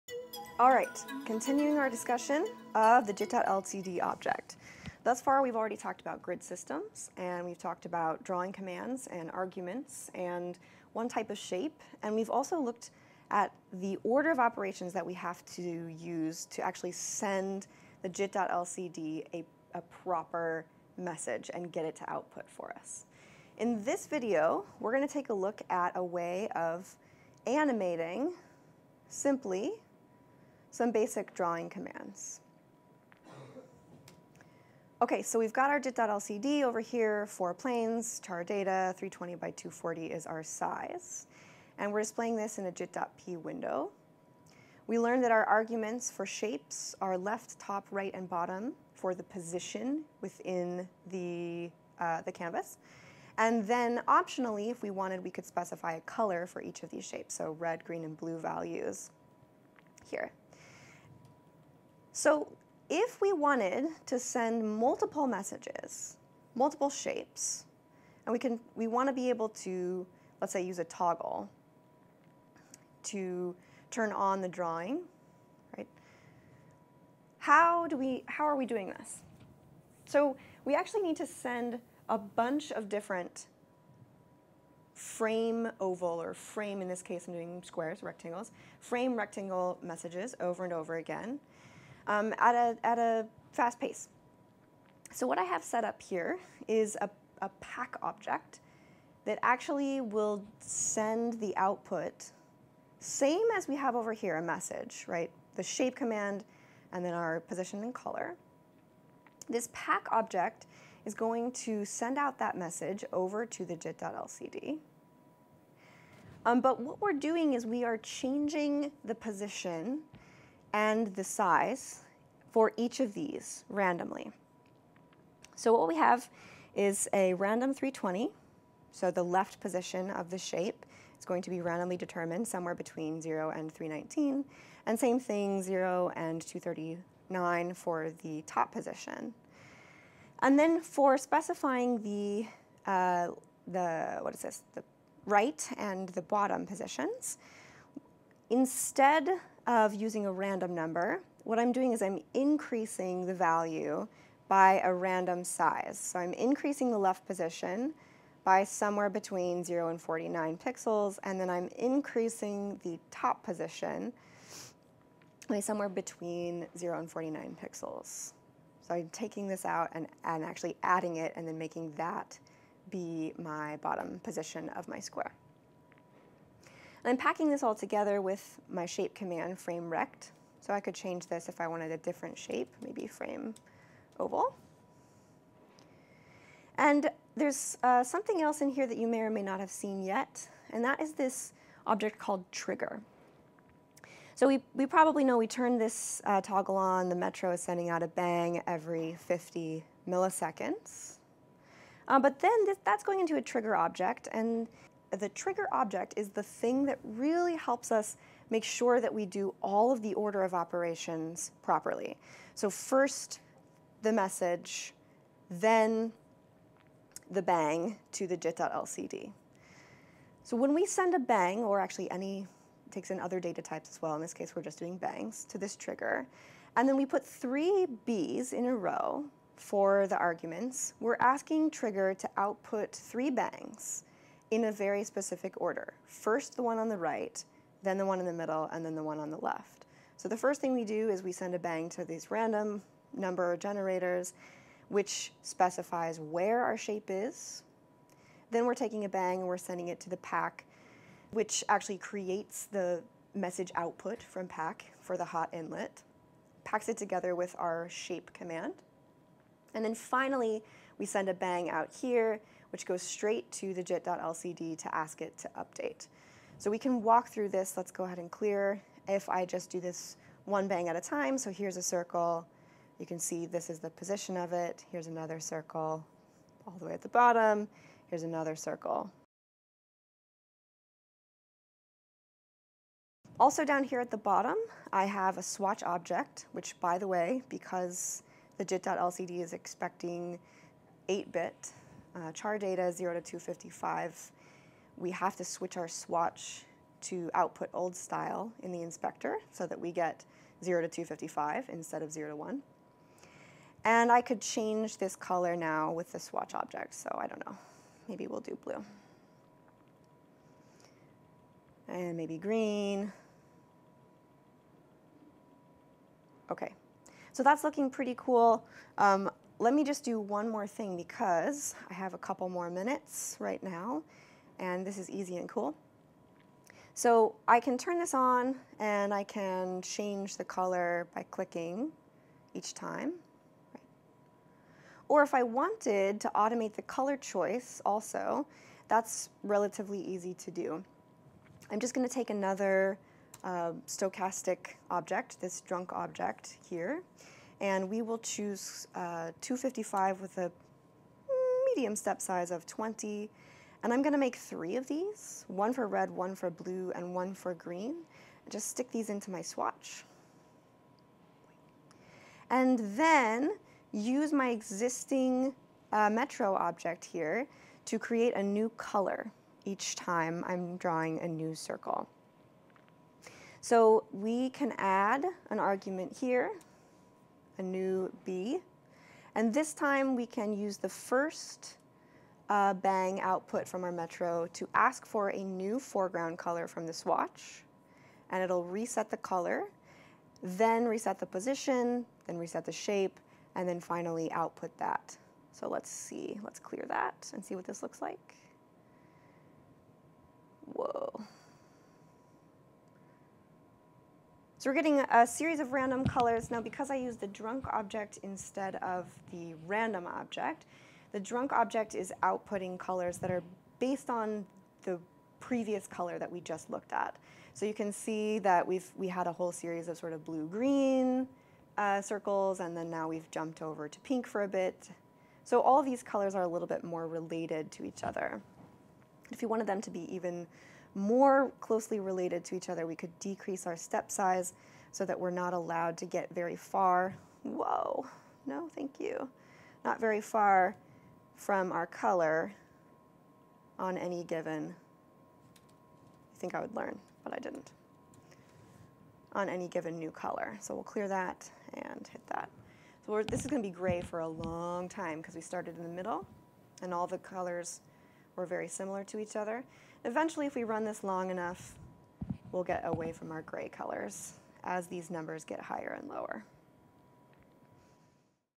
Jit.lcd basics, part 4 - Animation with a metro and toggle
2d animation with shapes and random values, using in jit.lcd object in Max 8.
Not actually so basic by this point in the playlist. See prior videos here: